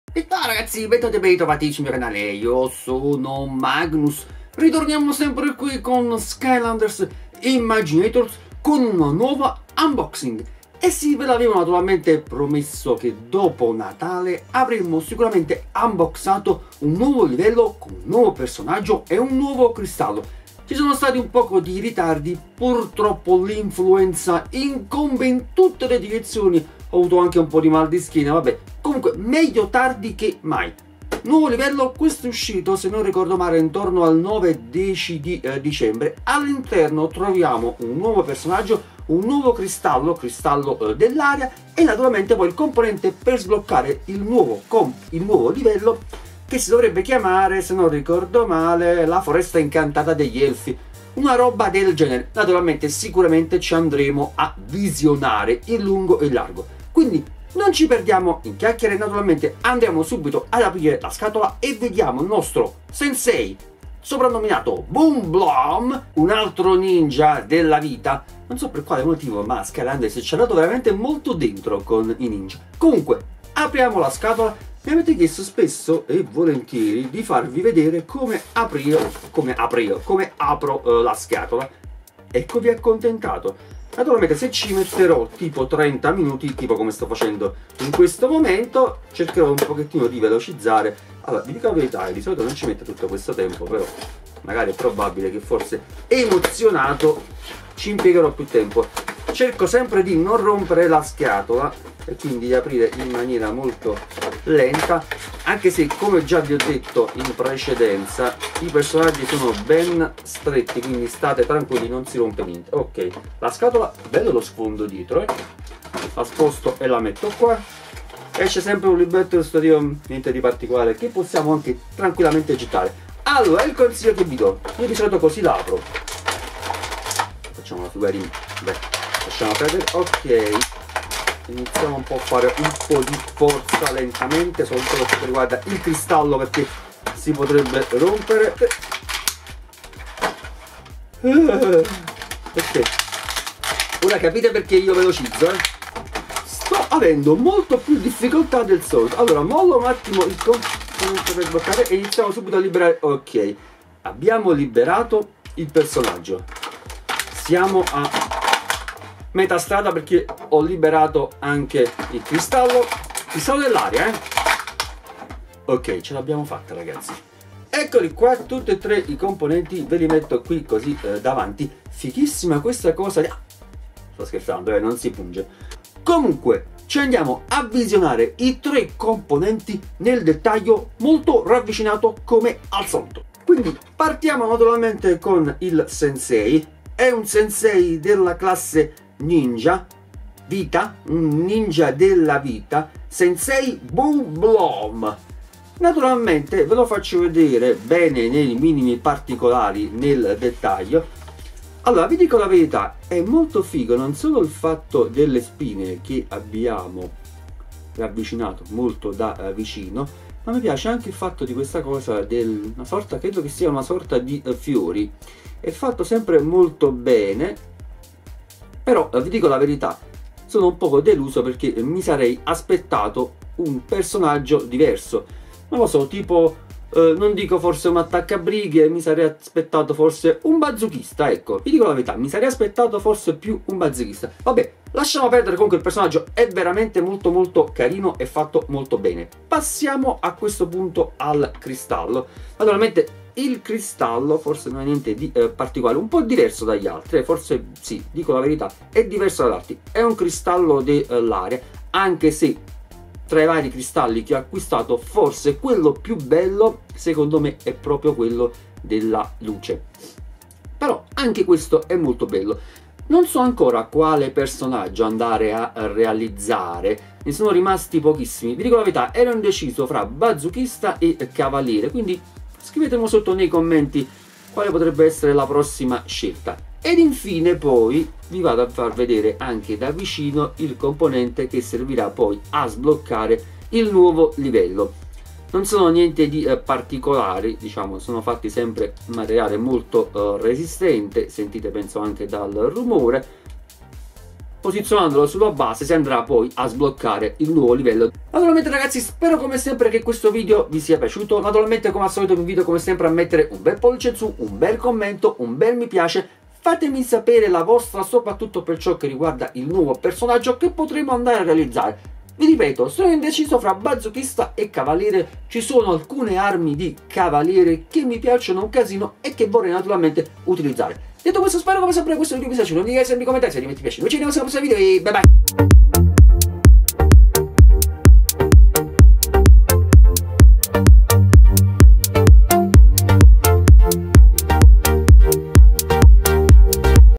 Skylanders Imaginators ITA - Nuovi Acquisti - Unboxing PS4
Skylanders Imaginators Unboxing ITA

TRAMA :
KAOS ha scoperto l'antico potere della Magia Mentale e lo sta usando per creare un inarrestabile esercito di Doomlanders! Per fermarlo, Eon ha chiamato alle armi tutti i Padroni dei Portali e i più grandi guerrieri mistici delle Skylands: i Sensei. Usa il potere della tua immaginazione per creare Skylanders personalizzati e combatti per difendere le Skylands al fianco dei potentissimi Sensei.

I miei video sono protetti da Copyright e ContentID.